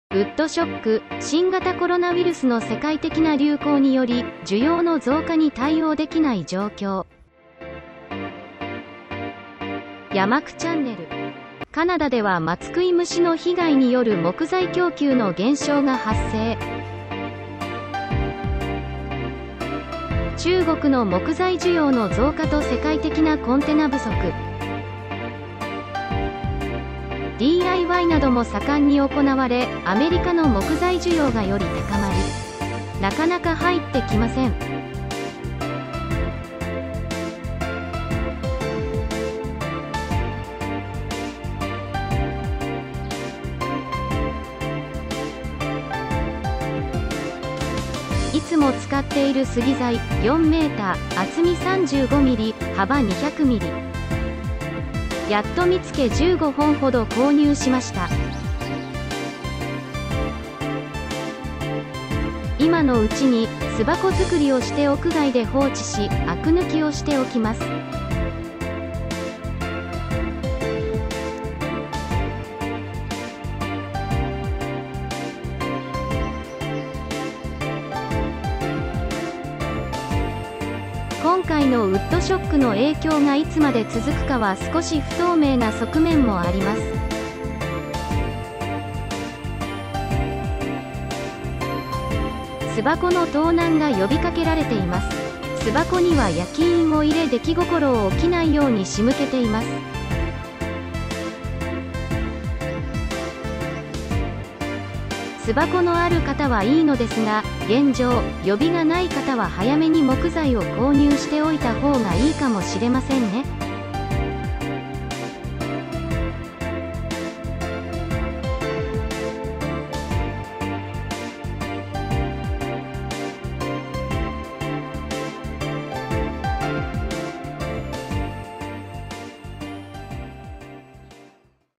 【 ニホンミツバチ 】巣箱に使う木材がなかなか入って来ません。巣伸びしているこの時期に継箱がないと困ります！
二ホンミツバチ　巣箱に使う木材　ウッドショック
新型コロナウィルスの世界的な流行により、需要の増加に対応できない状況
ホームセンターでいつも購入している木材がなかなか入って来ません。
巣箱のある方かたはいいのですが現状で予備が無い方は見かけたら購入しておいた方がいいかもしれませんね

ニホンミツバチ ミツバチ 日本ミツバチ 日本蜜蜂
スズメバチ 金稜辺 和歌山 紀の川市